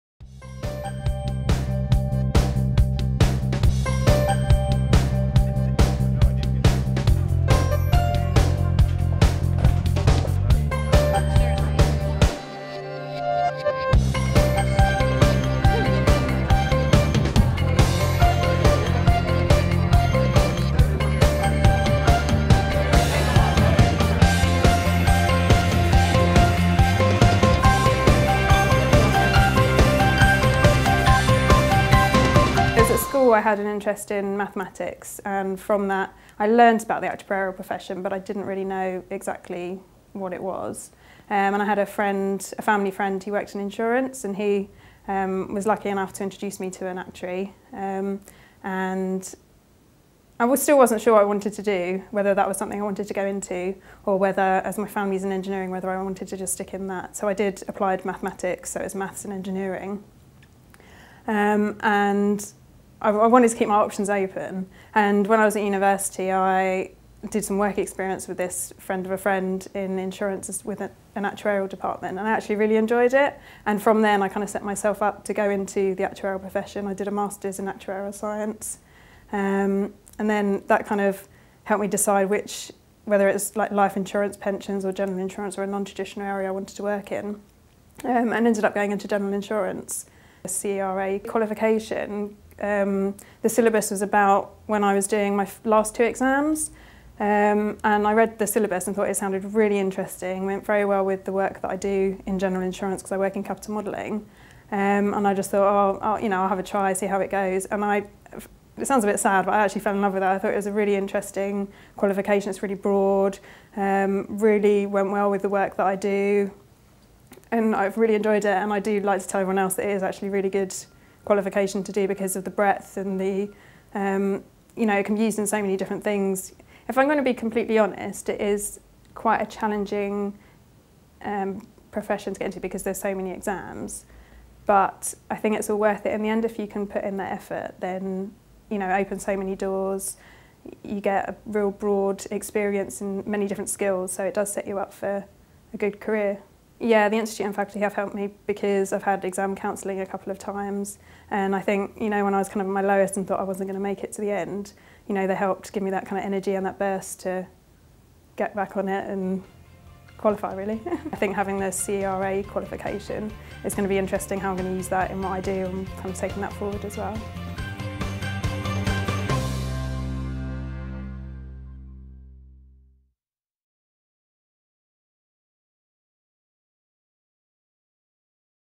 Ruth Ward talking about the CERA qualification
Ruth Ward talking about the CERA qualification at the New Qualifiers' ceremony in October 2011